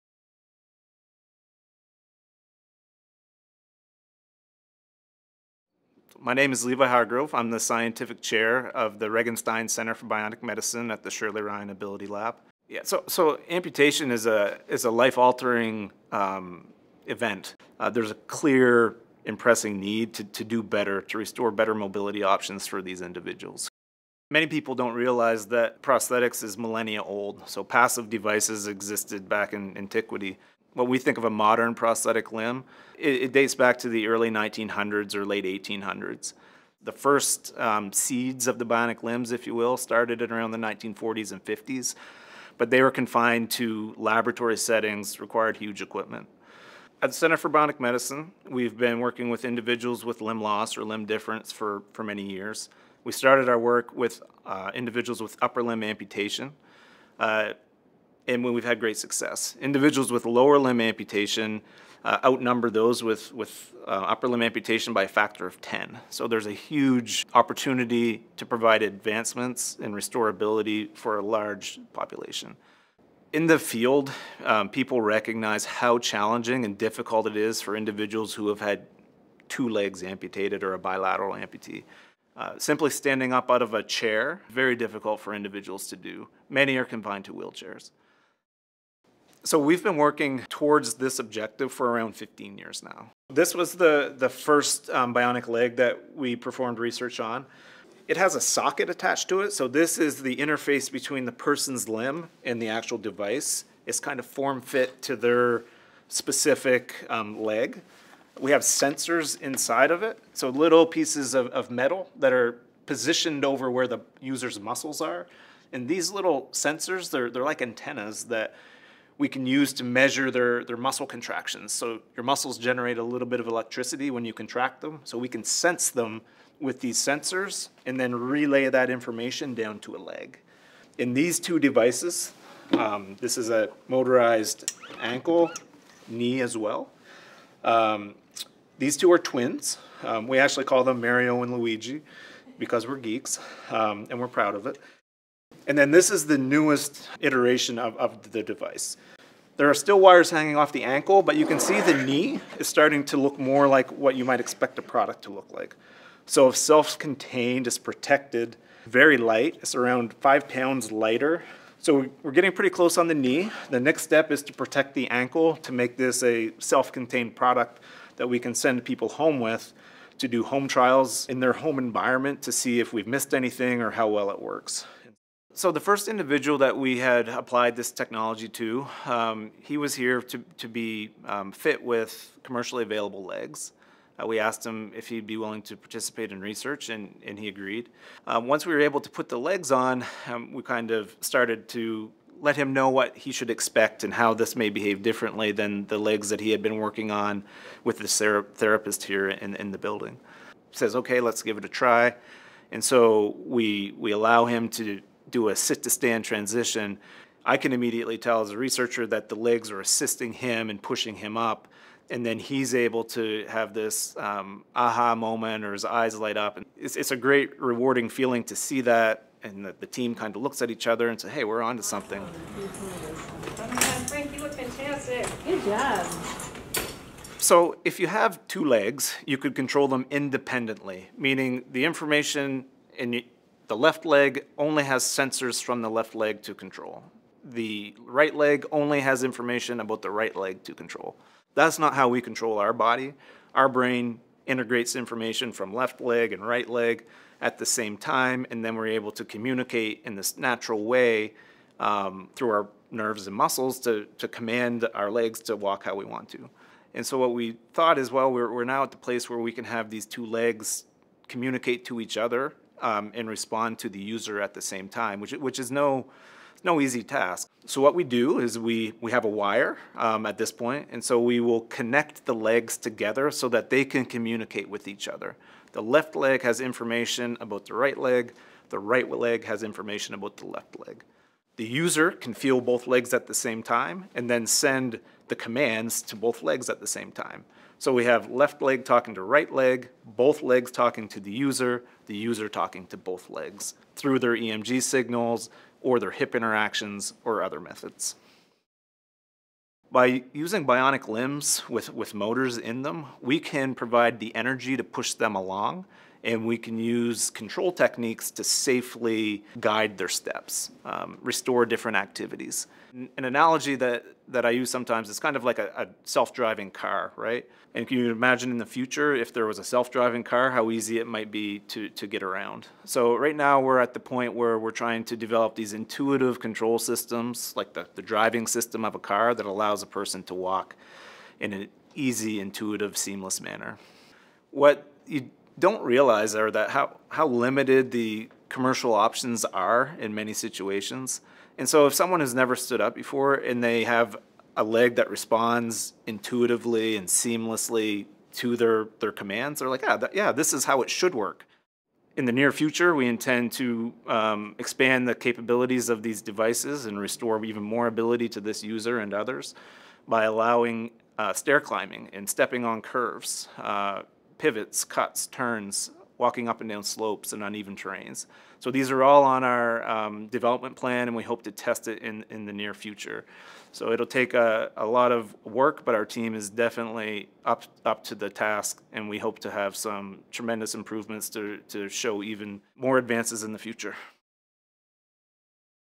Bionic Legs for Bilateral Above-Knee Amputations
The Regenstein Foundation Center for Bionic Medicine (CBM) has been developing coordinating bionic legs for people with dual above-knee amputations for more than a decade. This pair of bionic legs uses electromyography (EMG) — a technique for evaluating and recording the electrical activity produced by skeletal muscles — to enable the legs to “talk to each other,” thereby coordinate walking and other movement. Currently, these legs are only available in the research sector but we are hopeful that they will be commercially available in years to come.